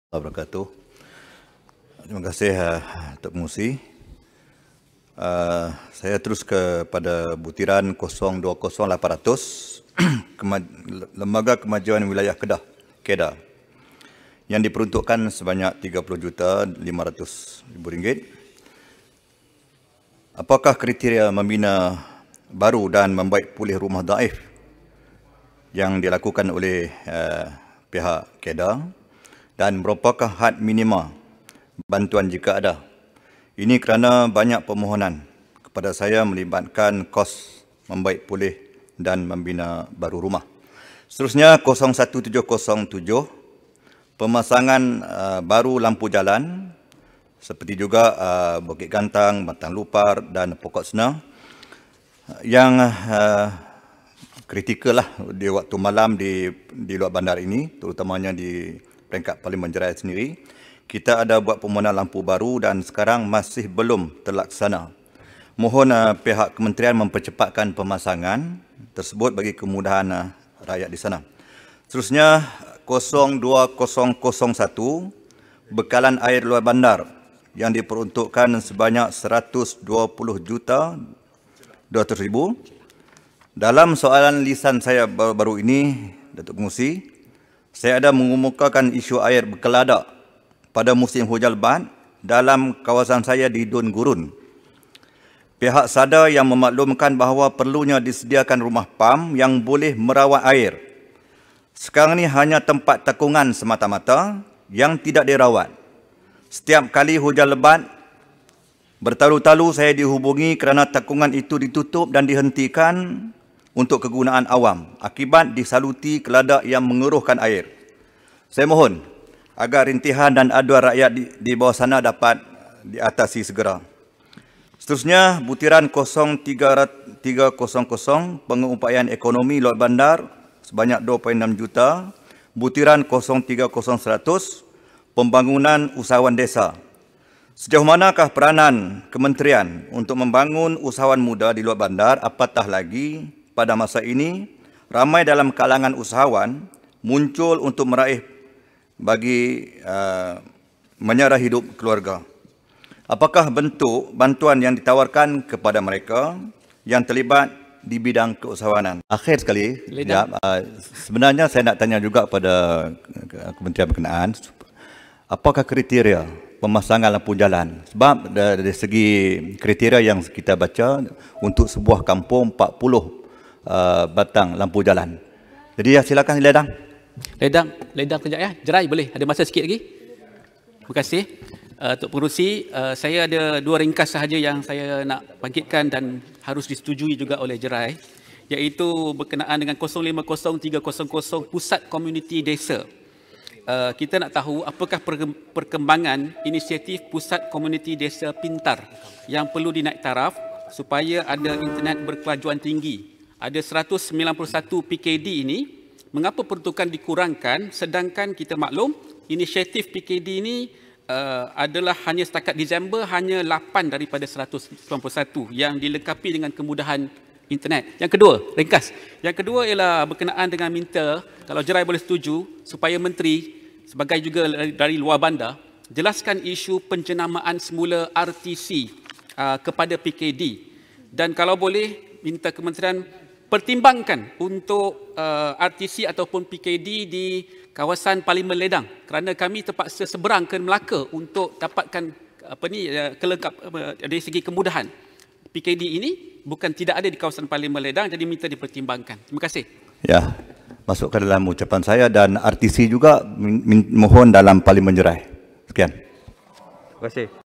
Isu Pembangunan Luar Bandar: YB Jerai Bahas Belanjawan 2020 Peringkat Jawatankuasa
2 Disember 2020 | Bajet KPLB Peringkat Jawatankuasa

Ahli Parlimen Jerai, YB Ustaz Sabri Azit bahas Rang Undang-Undang Perbekalan 2021 Peringkat Jawatankuasa bagi Kementerian Pembangunan Luar Bandar.